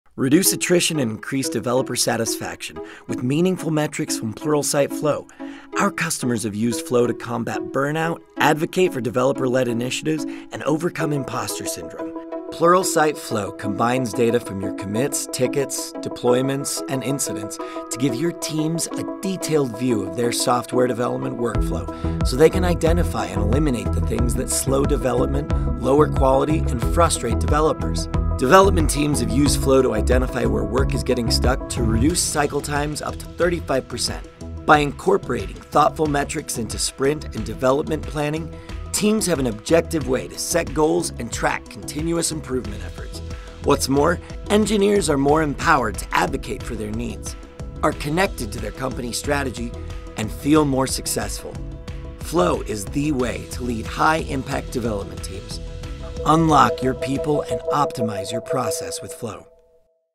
Pluralsight Flow is the way to build high impact development teams
Struggling to consistently deliver software? Your developers are battling unseen friction in your workflow. Reduce the friction, eliminate the bottlenecks, and shorten the cycle times by 35% with Pluralsight Flow.